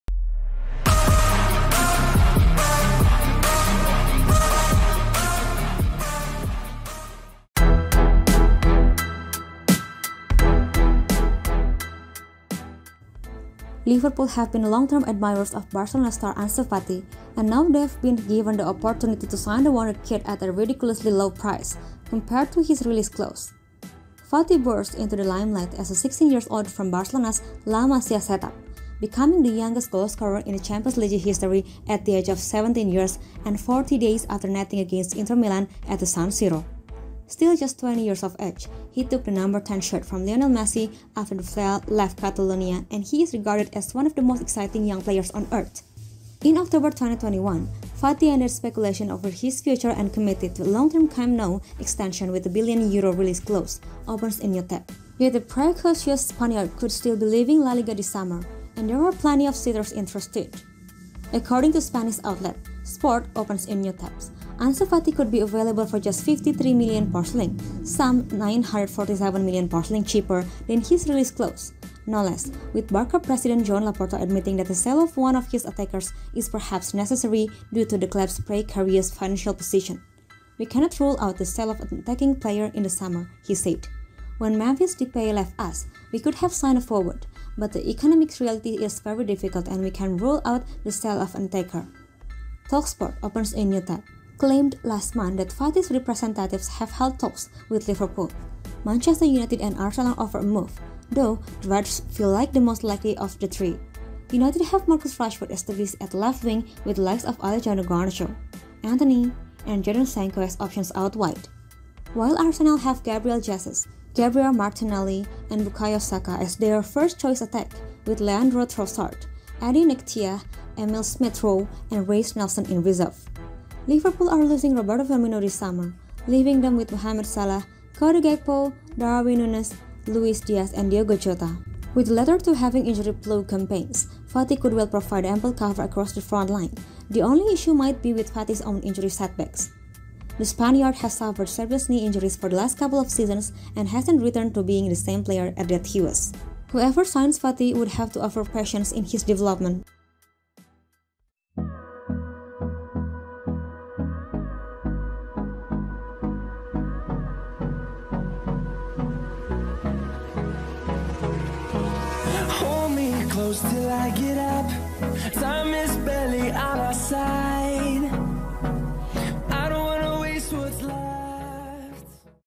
DEAL CLOSE: Barcelona To Accept £53m Bid For The Wonderkid This Summer
Liverpool have been long-term admirers of Barcelona star Ansu Fati – and now they've been given the opportunity to sign the wonderkid at a ridiculously low price, compared to his release clause.

Fati burst into the limelight as a 16-year-old from Barcelona's La Masia set-up, becoming the youngest goalscorer in Champions League history at the age of 17 years and 40 days after netting against Inter Milan at the San Siro. Still just 20 years of age, he took the No.10 shirt from Lionel Messi after the Flea left Catalonia and he is regarded as one of the most exciting young players on Earth.